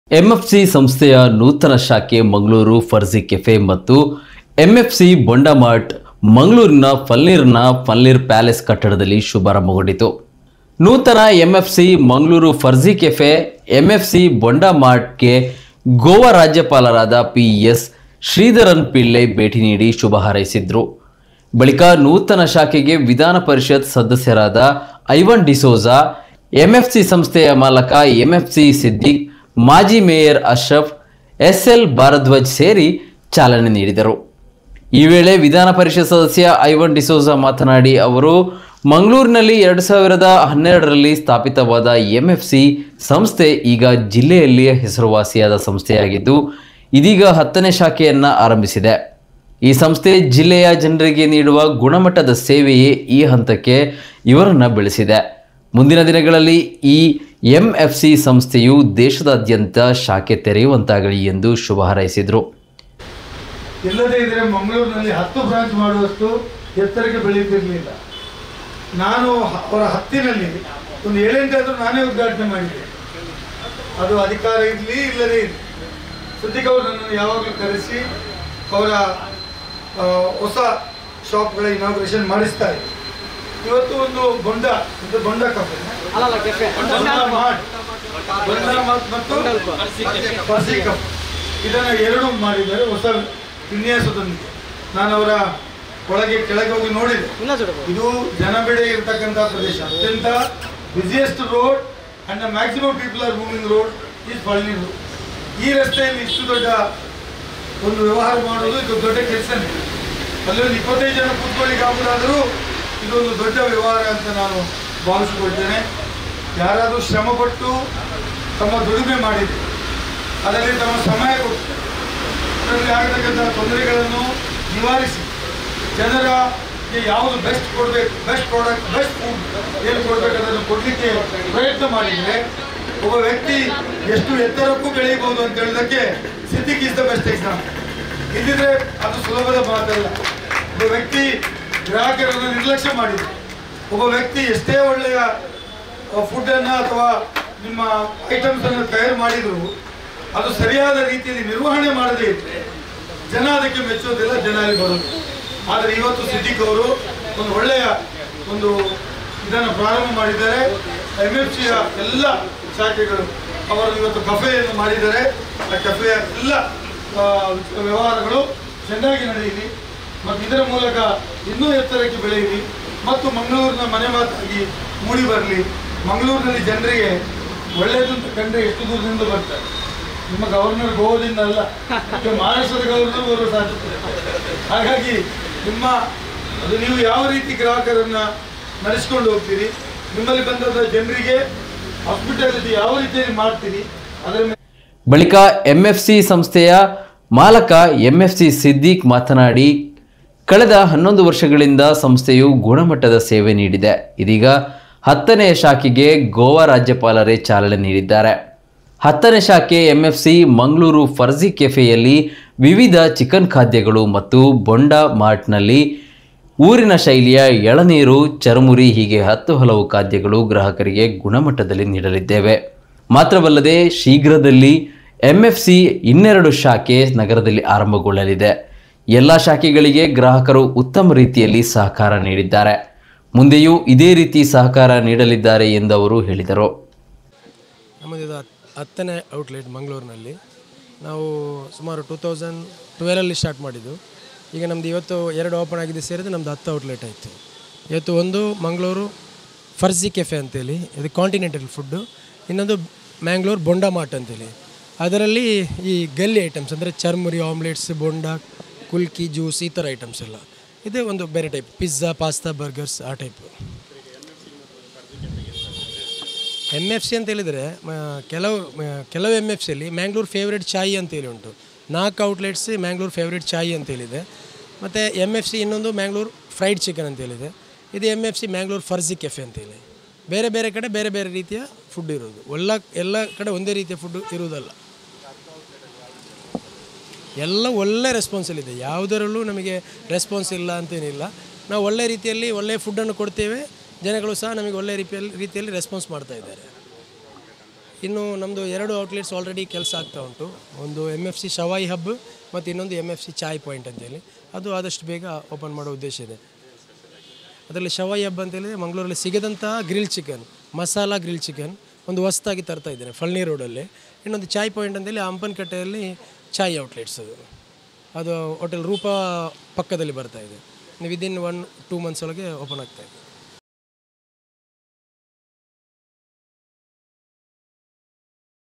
MFC ಸಂಸ್ಥೆಯ ನೂತನ ಶಾಖೆ ಮಂಗಳೂರು ಫರ್ಝಿ ಕೆಫೆ - ಫಳ್ನೀರ್ ಪ್ಯಾಲೇಸ್ ಕಟ್ಟಡದಲ್ಲಿ ಶುಭಾರಂಭ
ಎಂಎಫ್ಸಿ ಸಂಸ್ಥೆಯ ನೂತನ ಶಾಖೆ ಮಂಗಳೂರು ಫರ್ಝಿ ಕೆಫೆ, ಎಂಎಫ್ಸಿ ಬೊಂಡಾ ಮಾರ್ಟ್ ಮಂಗಳೂರಿನ ಫಳ್ನೀರ್ನ ಫಳ್ನೀರ್ ಪ್ಯಾಲೇಸ್ ಕಟ್ಟಡದಲ್ಲಿ ಶುಭಾರಂಭಗೊಂಡಿತು.